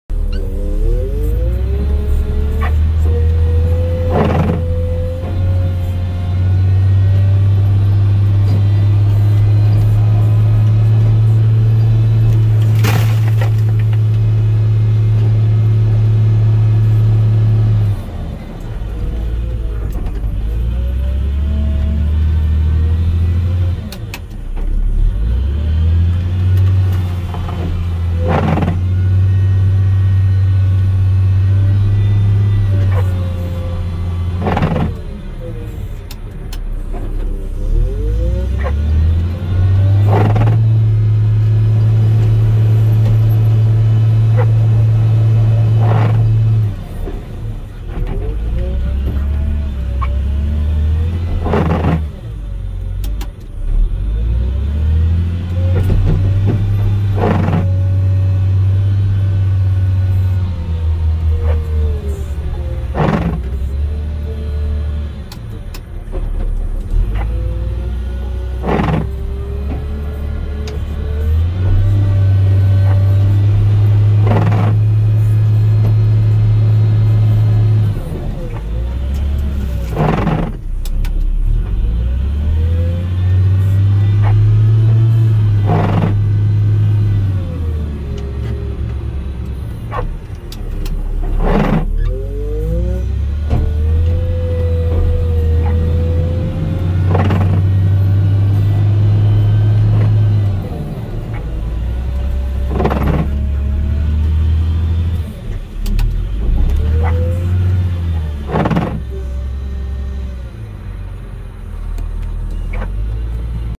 Snow Plowing After A Lot Of Rain Turning Into A Slush
We had a snow storm turned into freezing rain and then lots of rain.